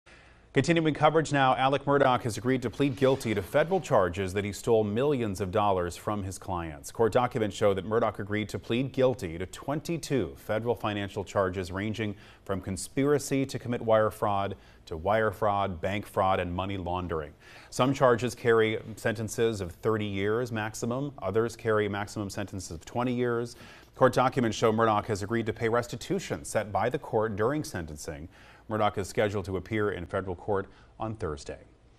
Murdaugh agrees to plea deal on federal financial charges
Alex Murdaugh has agreed to plead guilty to federal charges that he stole millions of dollars from his former clients.